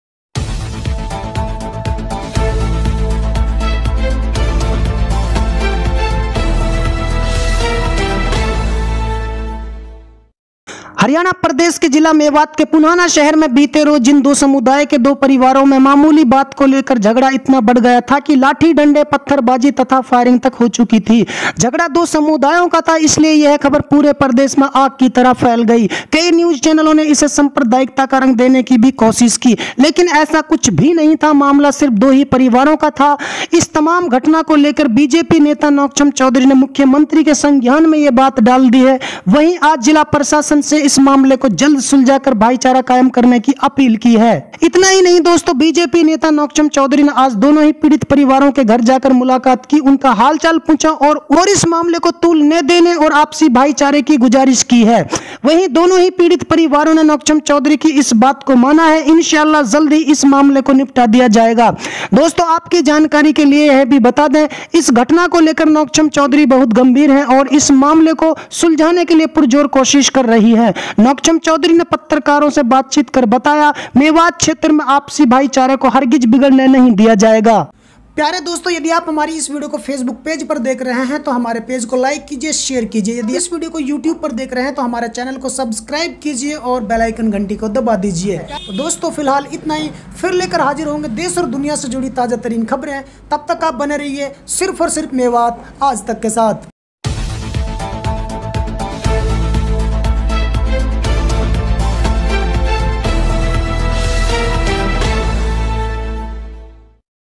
पुनहाना मामले की बड़ी खबर, मामले की निष्पक्ष जांच हो, नौक्षम चौधरी
punhana news ki taaja khabar Aaya naya mod